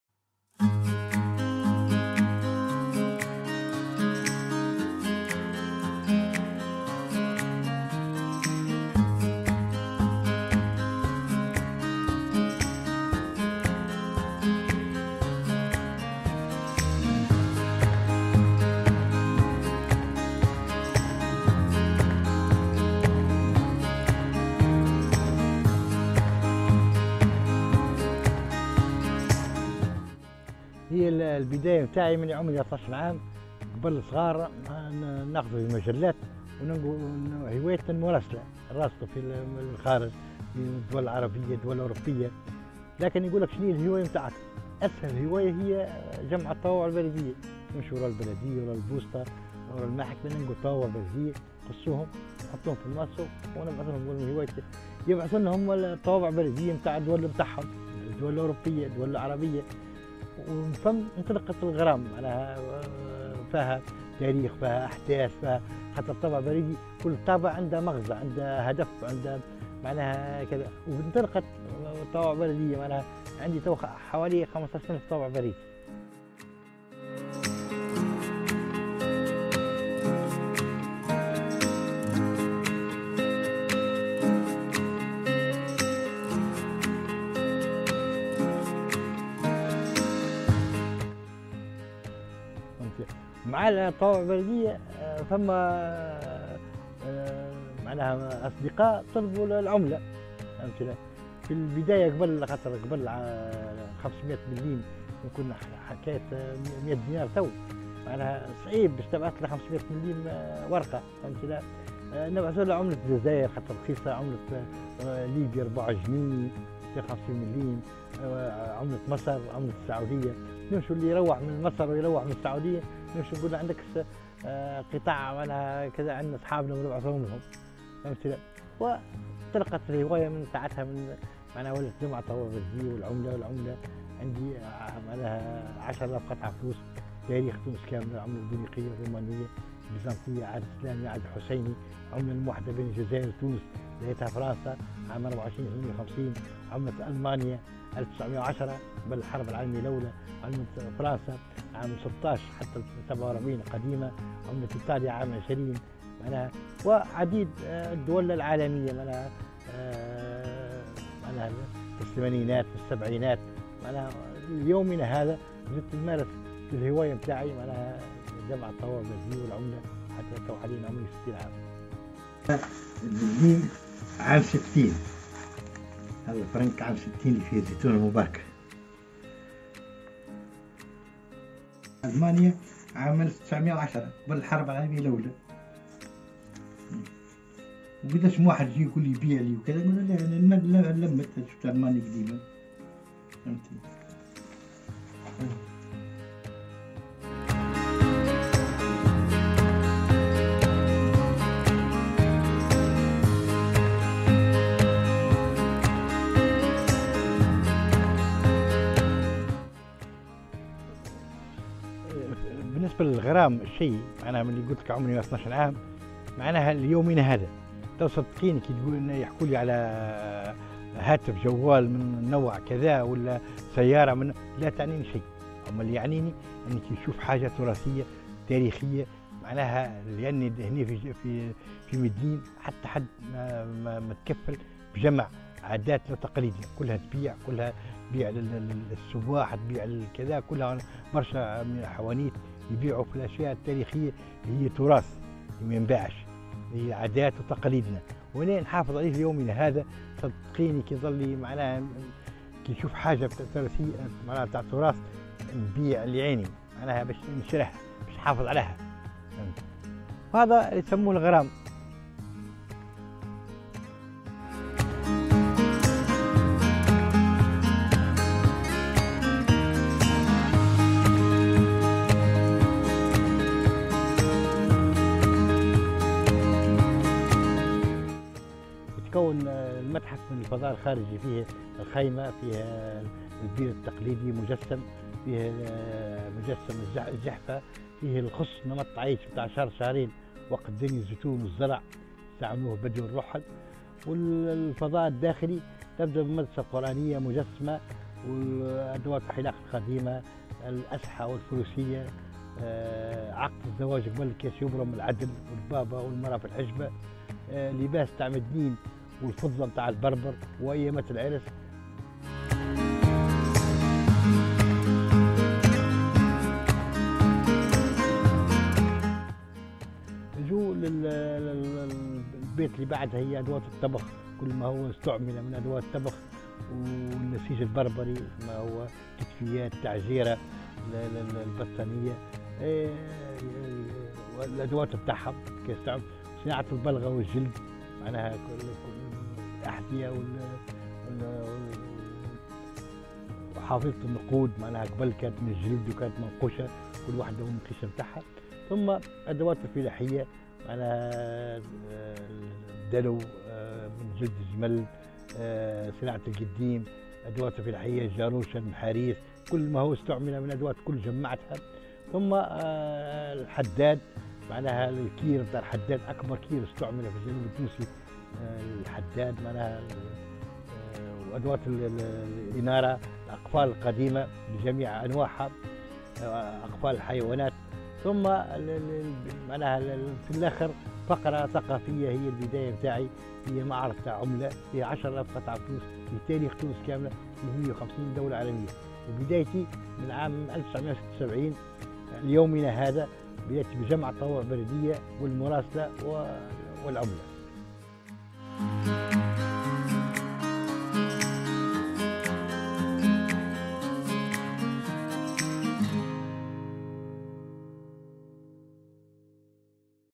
تونسي يحوّل غرف متحف أثري إلى كنز تراثي
أكثر من 40 سنة في ممارسة شغفه.. تونسي يحوّل غرف متحف أثري إلى كنز تراثي